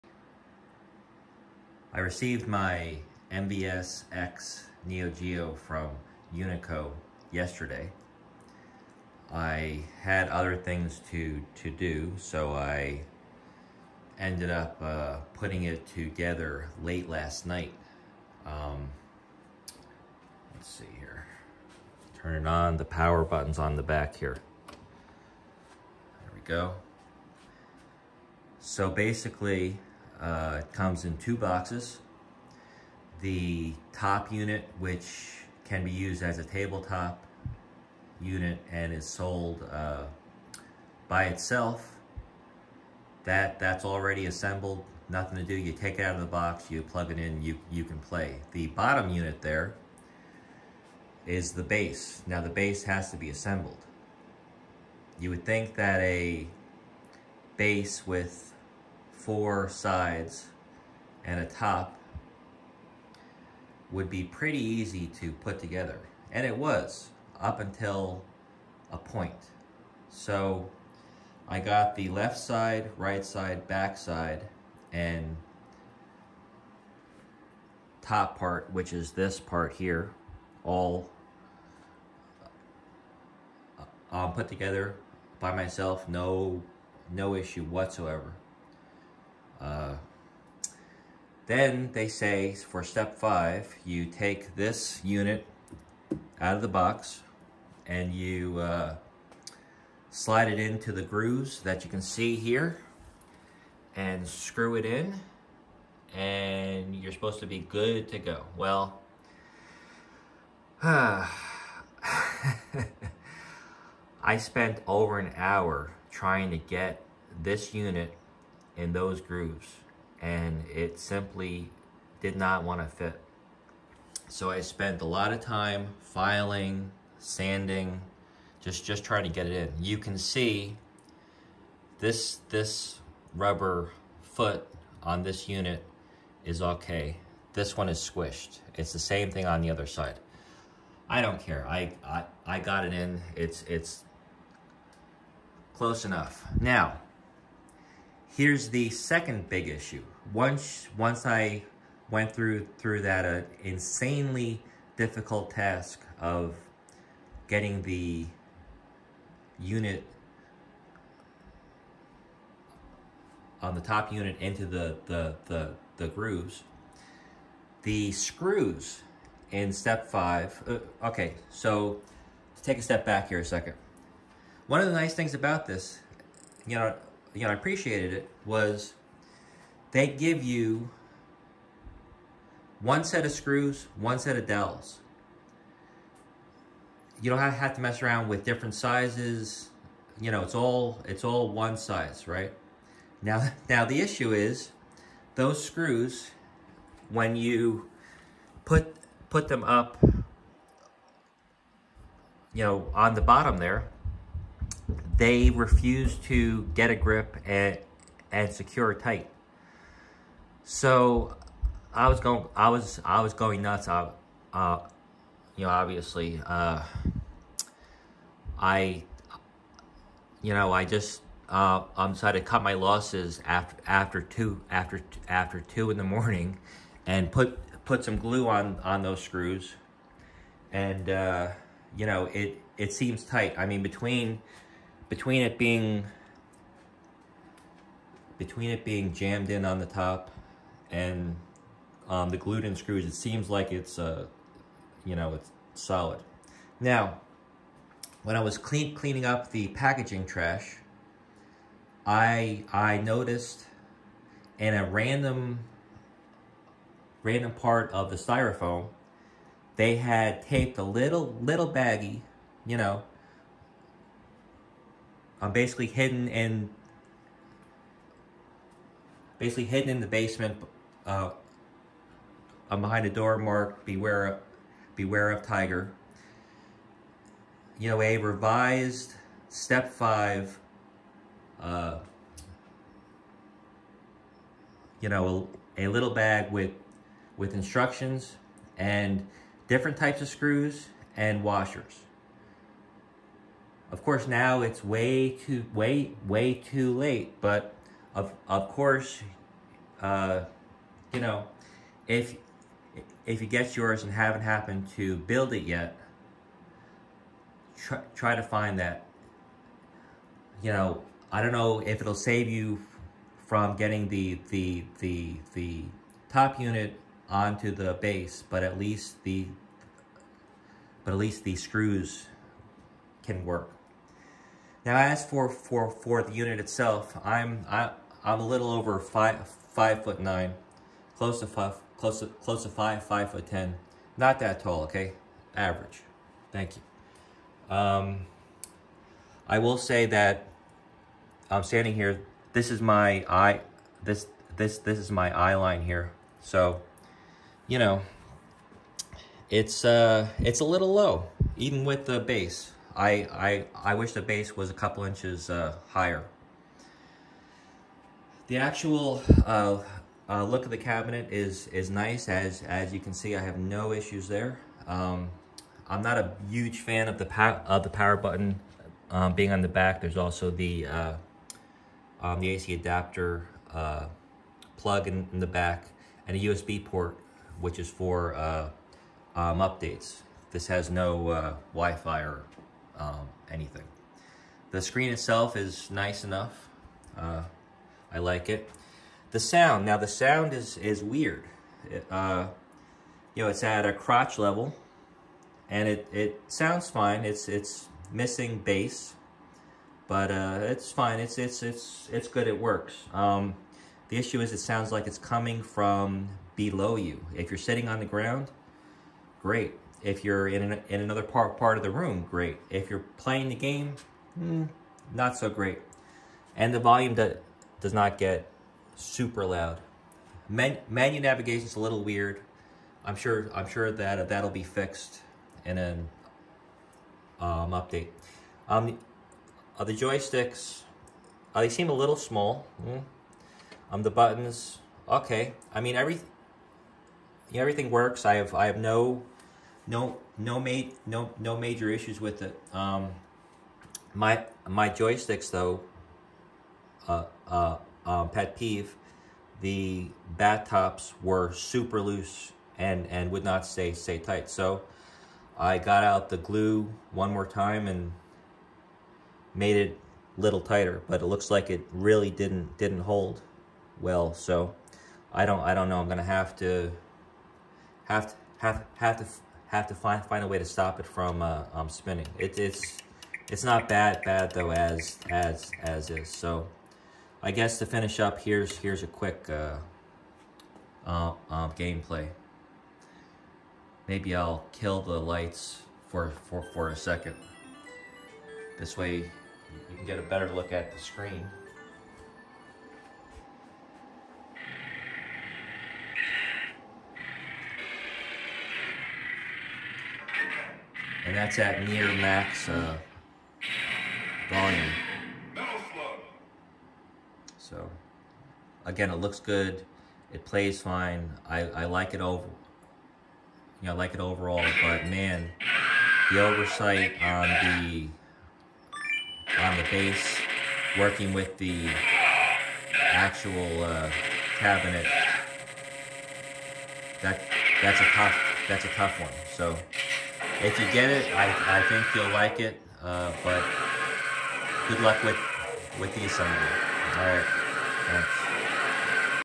UNICO MVSX (SNK) and Base are here - First impressions and a big warning!
My UNICO MVSX (SNK) and Base arrived yesterday. I spent late last night setting them up. There were two big issues that you need to watch out for! Check out the video for details, as well as this post on Armchair Arcade